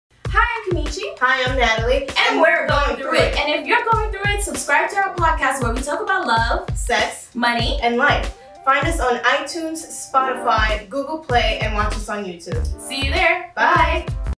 Going Through It Podcast
Just when you think you have things figured out, life loves to throw you a curveball, or at least that’s been our experience!

Being an adult is like being on a never ending roller coaster, full of ups and downs. While it can be a bumpy ride, you’re not alone. With “Going Through It”, Nat and Kenechi share their personal experiences growing up in NYC together. From relationships, sex, and breakups to finances and career advice, no topic is off-limits for this dynamic duo.

Our goal as we create this podcast is to share our experiences, struggles, and triumphs with you so that you can know you’re not alone and that we can all do this together.